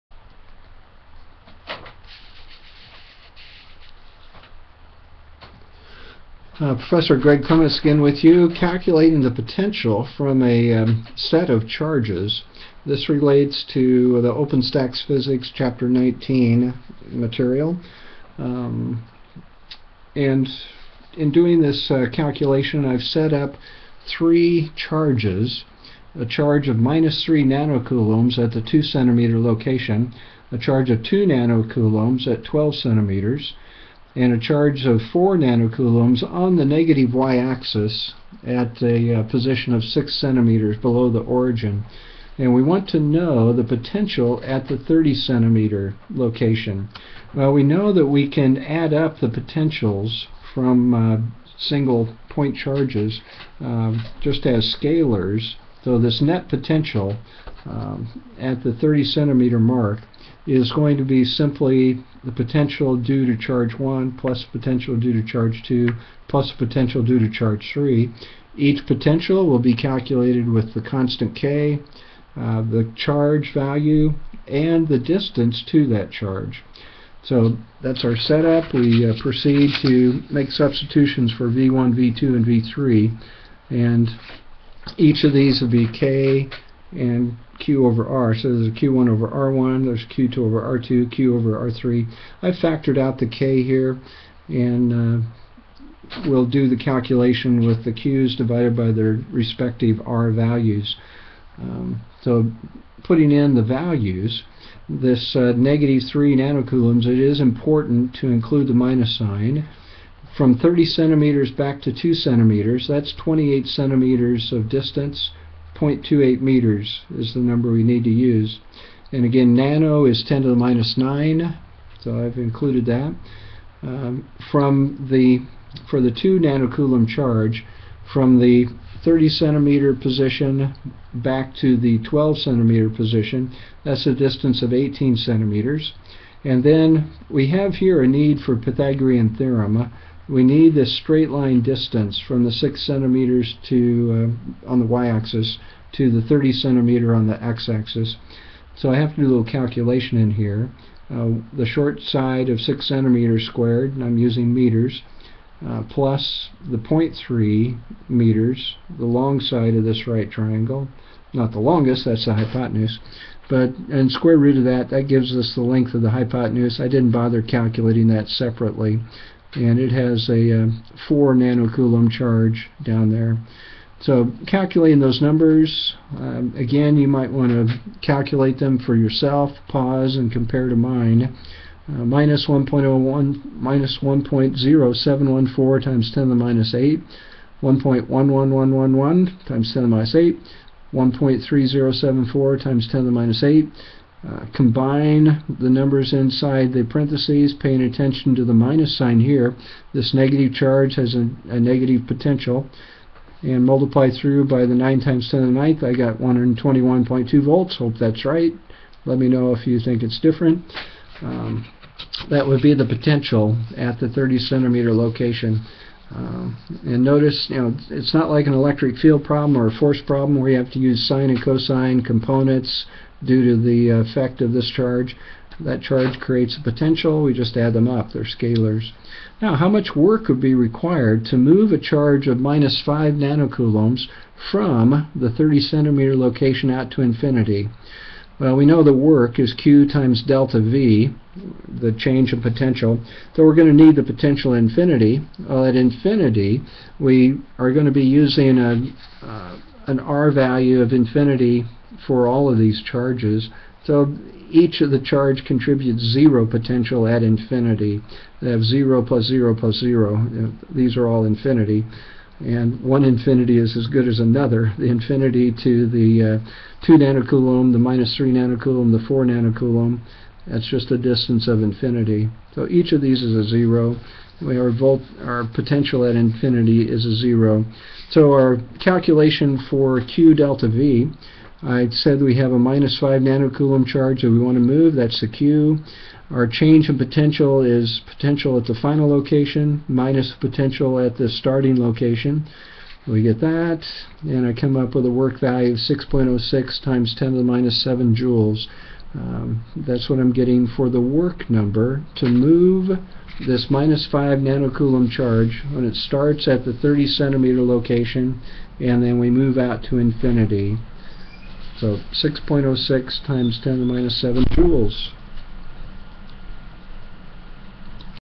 Electric Potential Due to 3 Charges and Work to Move another charge to infinity
Three charges are placed on a X Y coordinate system.  The net potential at a location is calculated.  The work to move a fourth charge from this location to infinity is calculated. Physics 2